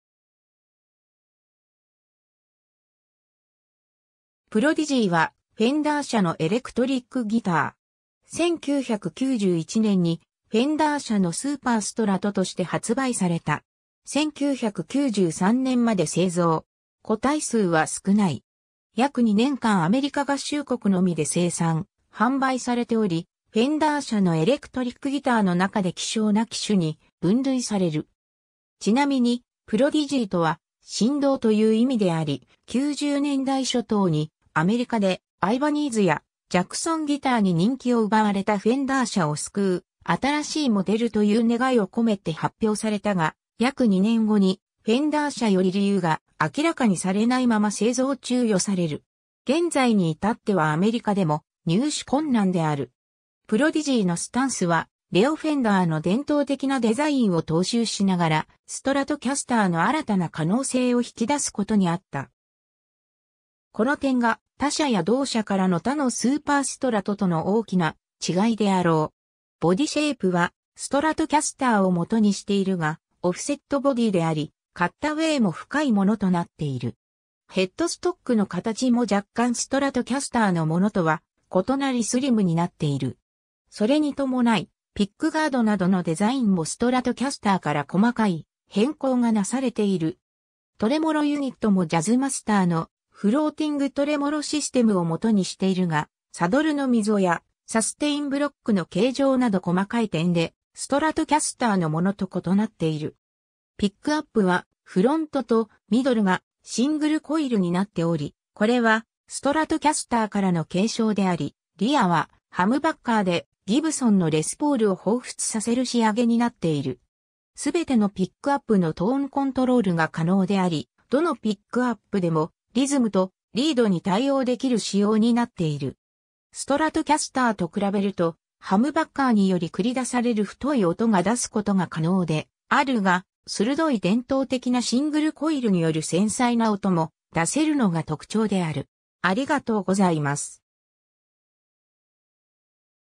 フェンダー・プロディジー
プロディジー(Prodigy)はフェンダー社のエレクトリックギター。
1991年にフェンダー社のスーパーストラトとして発売された。
1993年まで製造。
個体数は少ない。
 約2年間アメリカ合衆国のみで生産・販売されており、フェンダー社のエレクトリックギターの中で稀少な機種に分類される。
ちなみにProdigyとは「神童」という意味であり、90年代初頭にアメリカでアイバニーズやジャクソンギターに人気を奪われたフェンダー社を救う新しいモデルと言う願いを込めて発表されたが、約2年後にフェンダー社より理由が明らかにされないまま製造中止される。
現在にいたってはアメリカでも入手困難である。
 プロディジーのスタンスはレオ・フェンダーの伝統的なデザインを踏襲しながらストラトキャスターの新たな可能性を引き出すことにあった。
この点が他社や同社からの他のスーパーストラトとの大きな違いであろう。
 ボディシェイプはストラトキャスターを基にしているが、オフセットボディーであり、カッタウェイも深いものとなっている。
ヘッドストックの形も若干ストラトキャスターのものとは異なりスリムになっている。
それに伴いピックガードなどのデザインもストラトキャスターから細かい変更がなされている。
 トレモロ・ユニットもジャズマスターのフローティングトレモロシステムを基にしているがサドルの溝やサステインブロックの形状など細かい点でストラトキャスターのものと異なっている。
 ピックアップは、フロントとミドルがシングルコイルになっており、これはストラトキャスターからの継承であり、リアはハムバッカーでギブソンのレスポールを彷彿させる仕上げになっている。
全てのピックアップのトーンコントロールが可能であり、どのピックアップでもリズムとリードに対応できる仕様になっている。
ストラトキャスターと比べるとハムバッカーにより繰り出される太い音が出す事が可能であるが、鋭い伝統的なシングルコイルによる繊細な音も出せるのが特徴である。